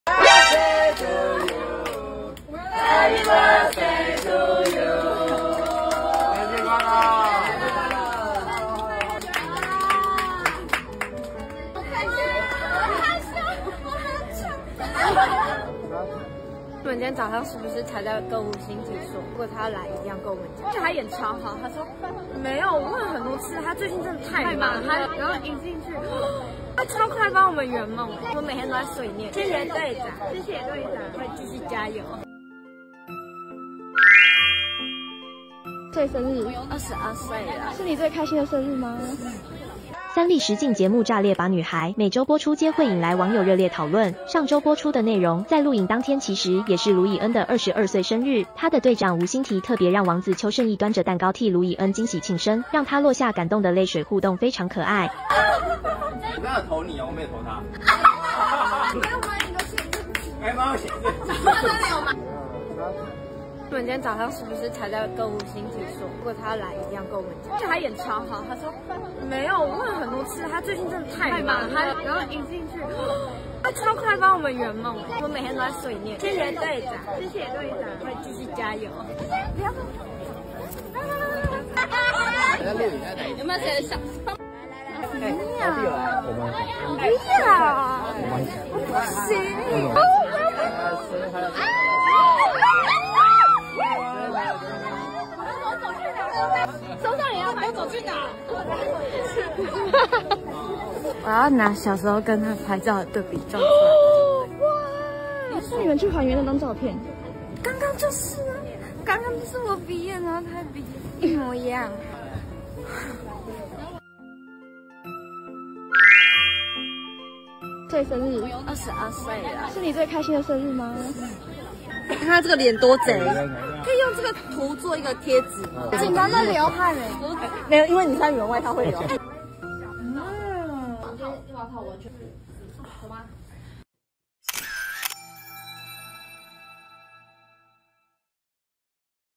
《炸裂吧》吳心緹助攻！盧以恩生日「見王子端蛋糕現身」害羞反應全曝光｜三立娛樂星聞
影片來源：IG/showupgirls_official